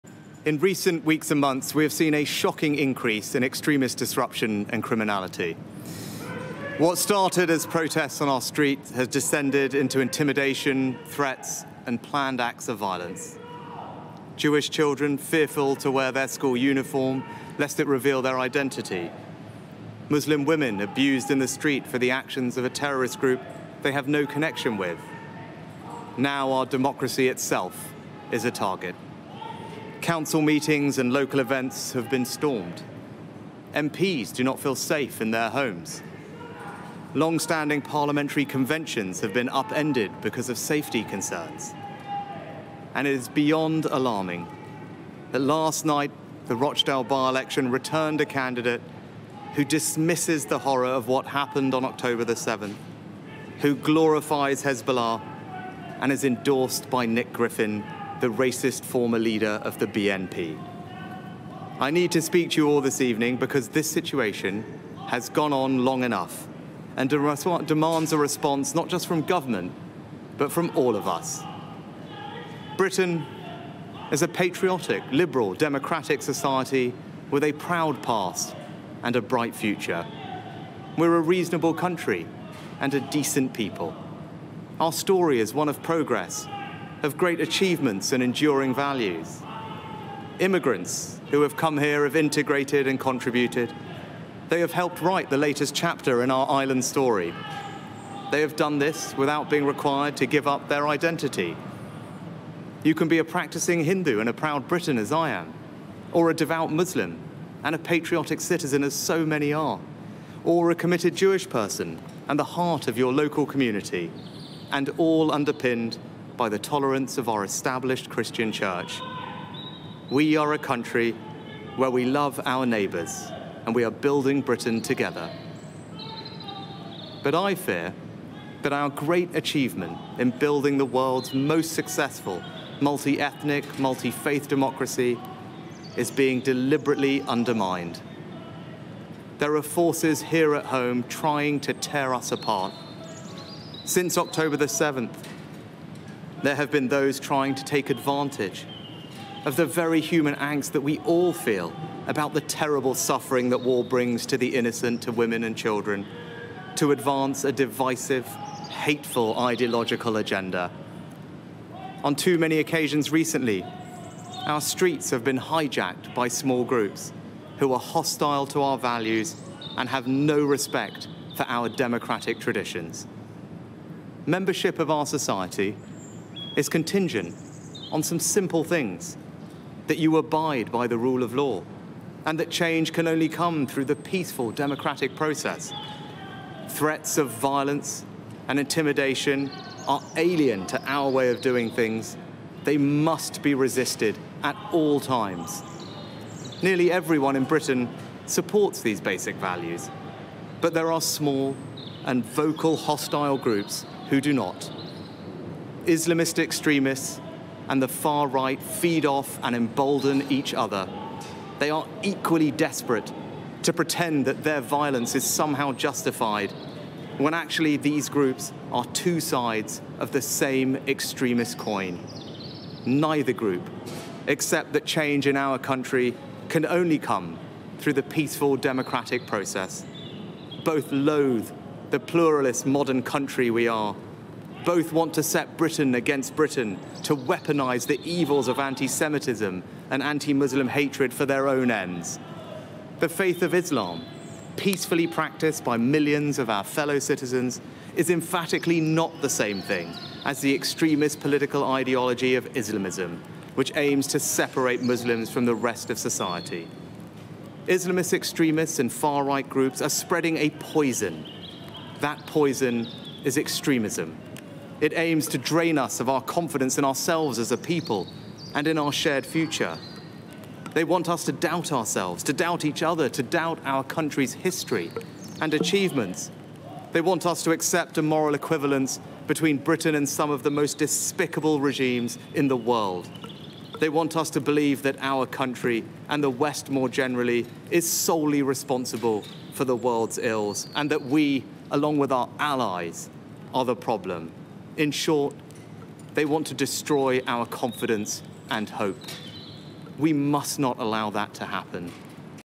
PM rails against 'extremist forces trying to tear us apart' in Downing Street address
The prime minister said there has been a "shocking increase in extremist disruption and criminality", adding that "now our democracy itself is a target".

Mr Sunak also described the result of the Rochdale by-election, which was won by controversial left-wing politician George Galloway, as "beyond alarming".